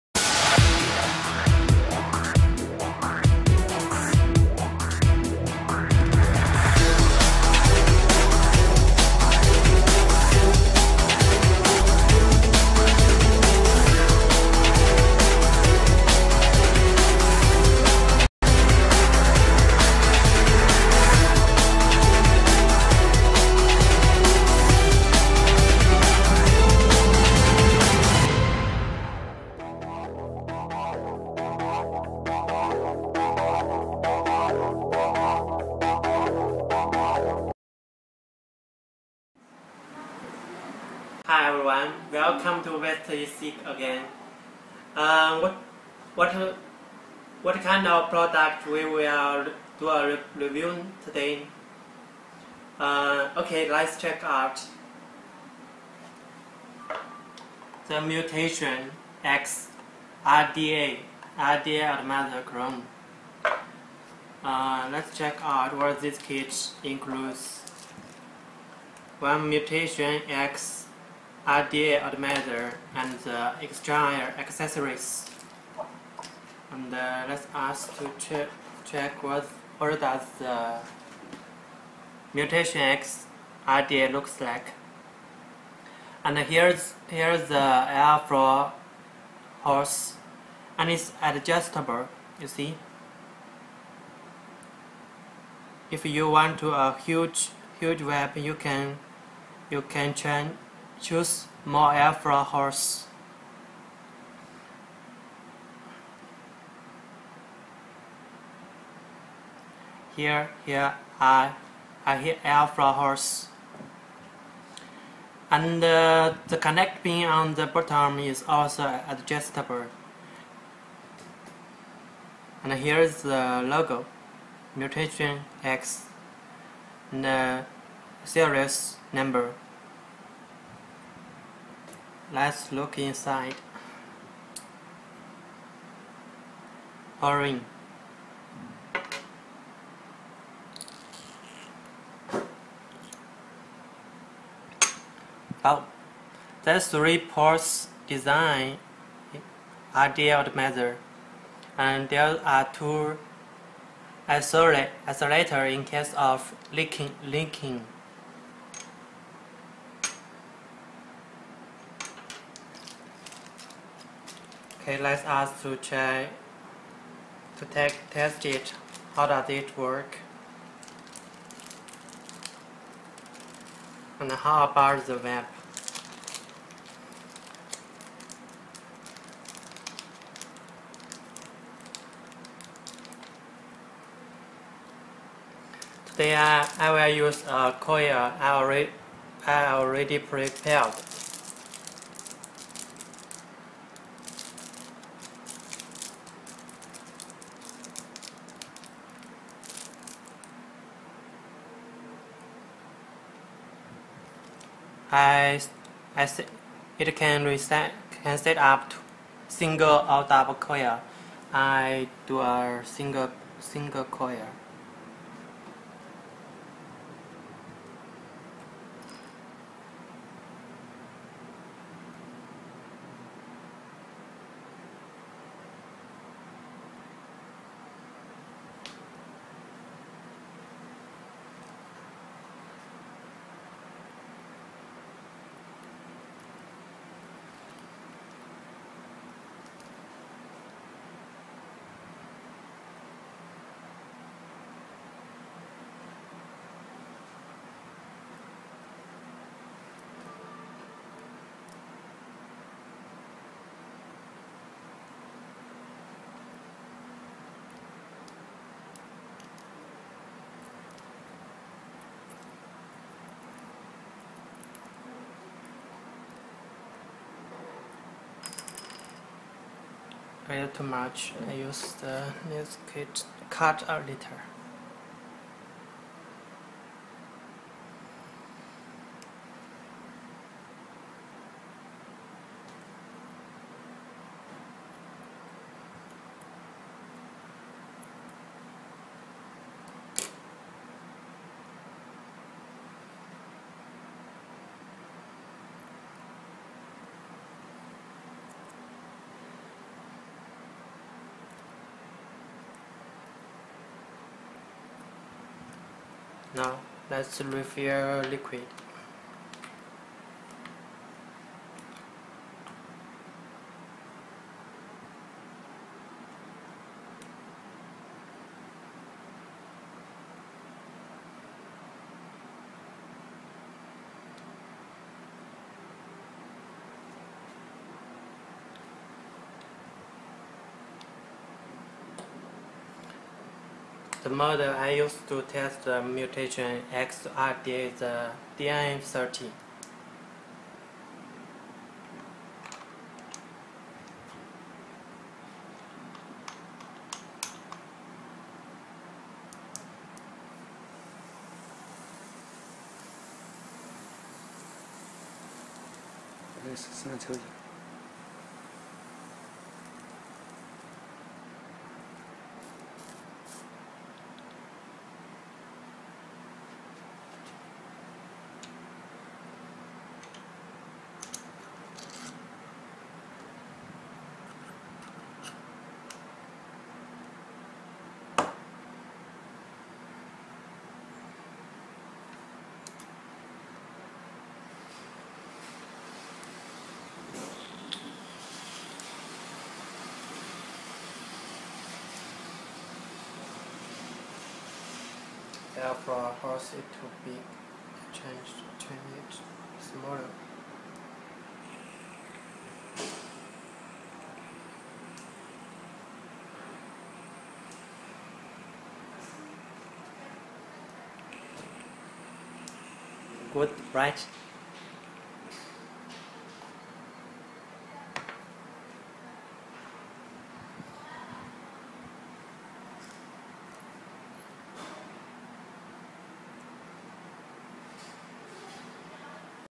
18 airflow holes mutation x rda atomizer clone review by wisterecig
The mutation x really a awsome rda atomizer,nice design,huge vapor.
1.High grade 306 stainless steel
2. total 18 airflow holes,and adjustable
3.Top Heatsink
4.3 posts,single and duble coil setup
5.Brass,ss,black three colors
6.Deep juice well